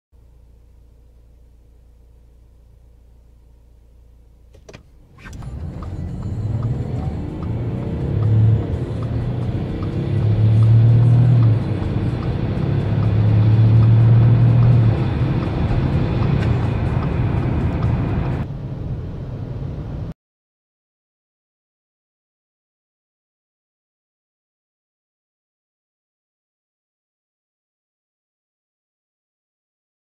Audi A4 B8 2.7 TDI VS. BMW 218d F45 Active Tourer - Acceleration 0-100km/h
Audi A4 B8 2.7 TDI
190HP
400Nm
0-100 in 8.7s

BMW 218d F45 Active Tourer
150HP
330Nm
0-100 in 8.6s